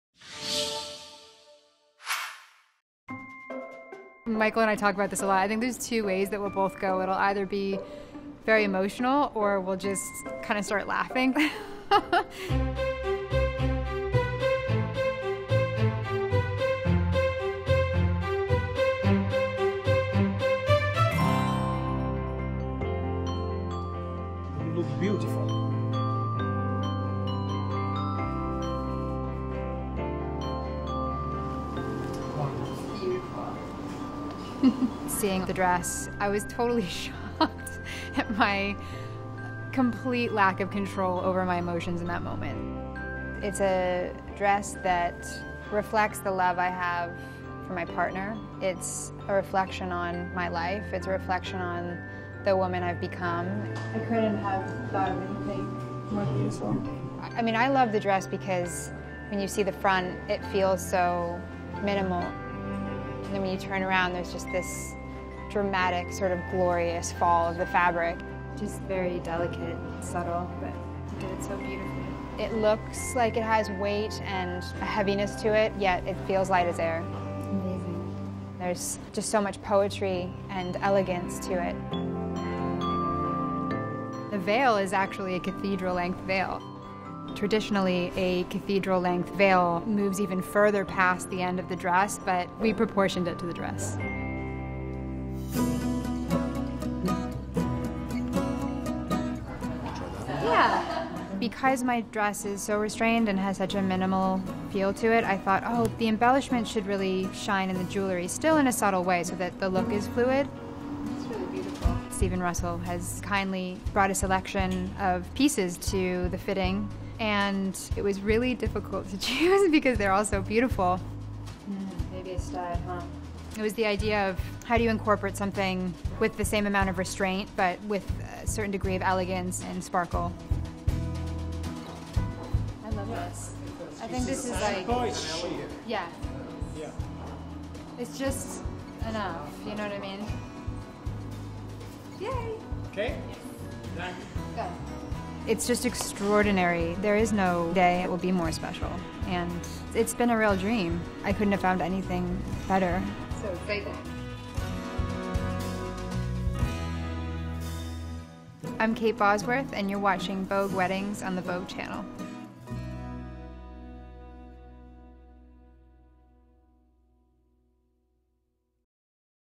Kate Bosworth Sees Her Oscar de la Renta Wedding Dress for the Very First Time - Vogue Weddings
Kate Bosworth returns to the Oscar de la Renta showroom in New York City to see her dress for the first time in the final fitting of her wedding gown paired with a dramatic cathedral veil. To balance the gorgeous simplicity of Oscar de la Renta’s custom gown, Kate tries on a selection of sparkling estate diamond jewelry to polish off her wedding day look.